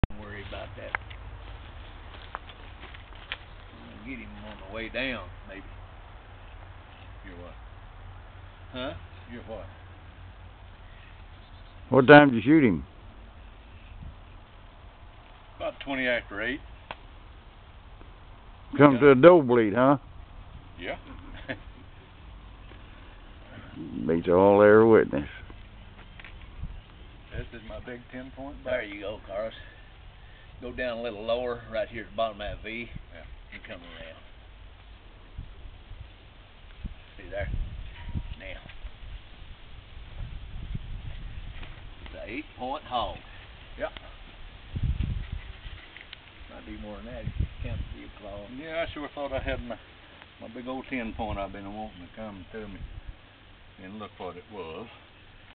Wild hog hunting
Wild boar hunting in Arkansas, responded to a doe bleat.